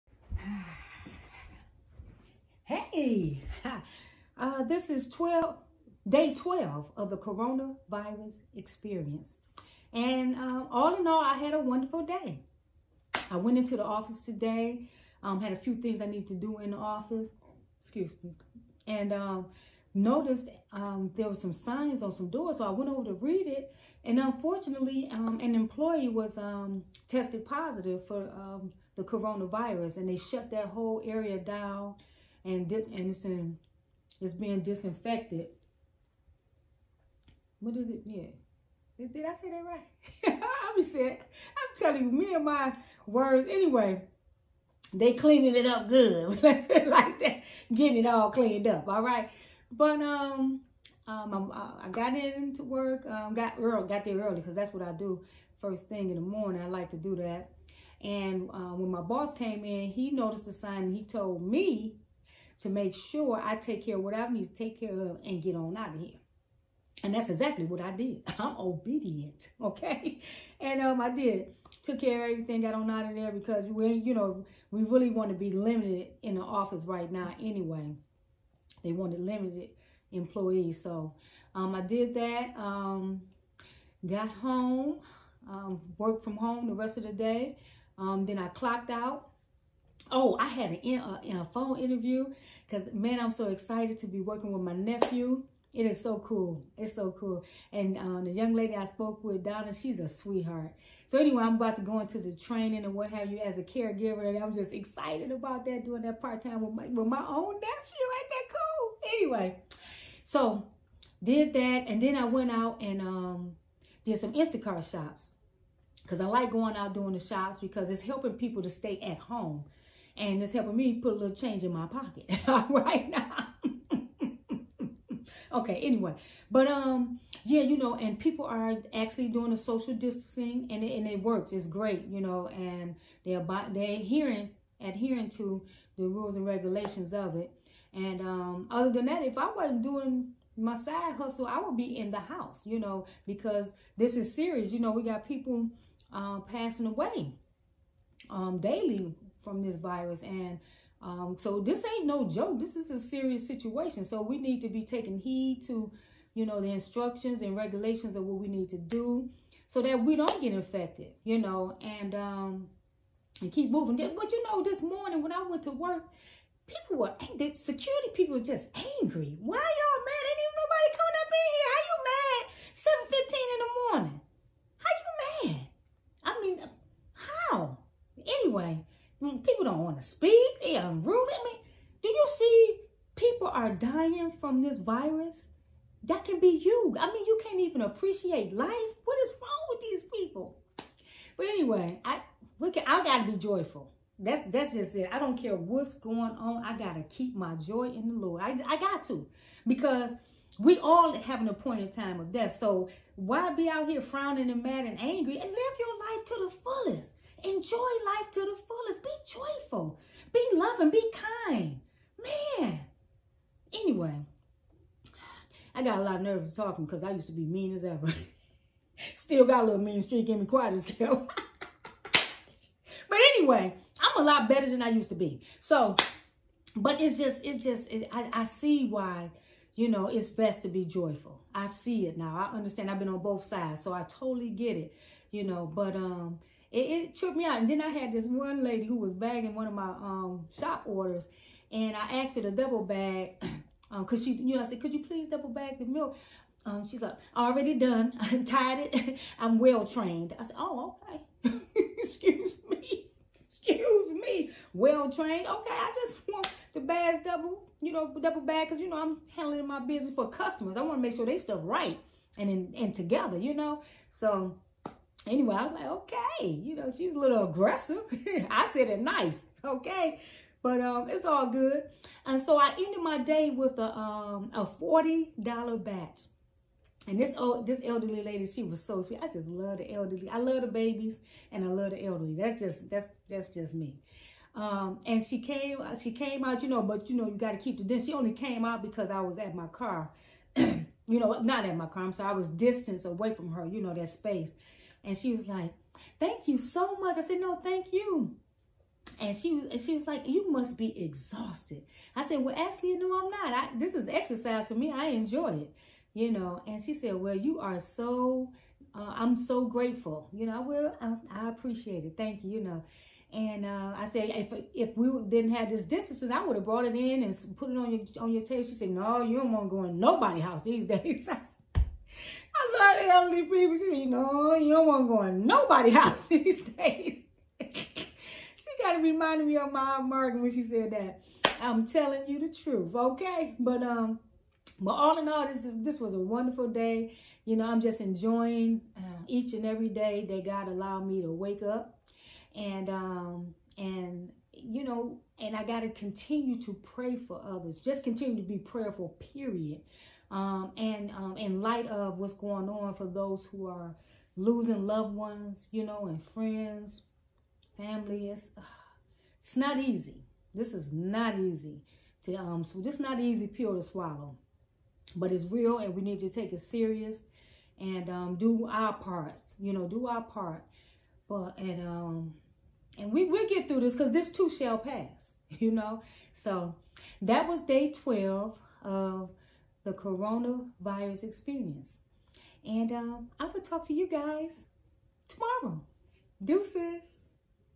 Day 12 Of The CoronaVirus Experience (Positive Test)
God Is Good! People...People...People! Positive test! Regardless I had A Blessed & Wonderful Day! Thanking God For Another Day! ❤️😇❤️😇❤️😇❤️😇❤️😇 TheMedicalMary Work from Home corona virus coronavirus instacart booming people need groceries lockdown manners positive test Focused on Enjoying everyday God allows me to wake up! Hallelujah!